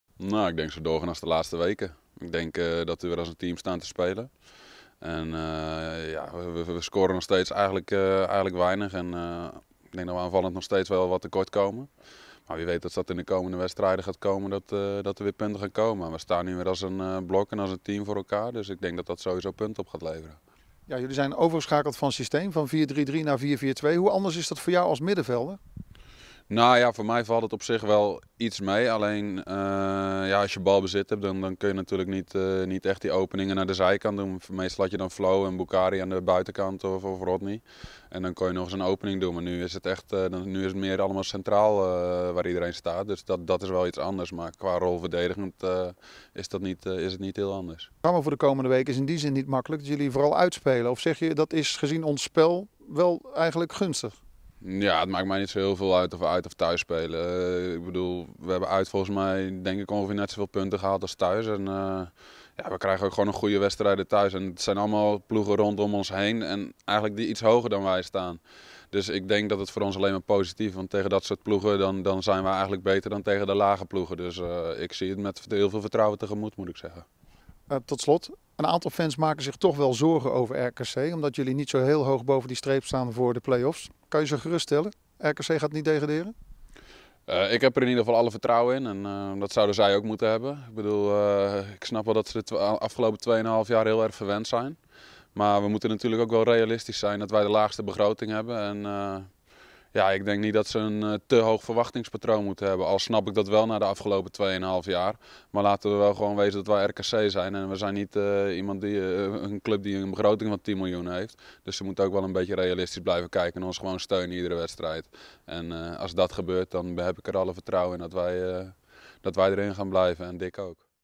Interview Sander Duits
Middenvelder Sander Duits sprak met RKC TV over het verloop van het seizoen tot nu toe én de weken die komen gaan. De speler van RKC WAALWIJK heeft alle vertrouwen in een goede afloop.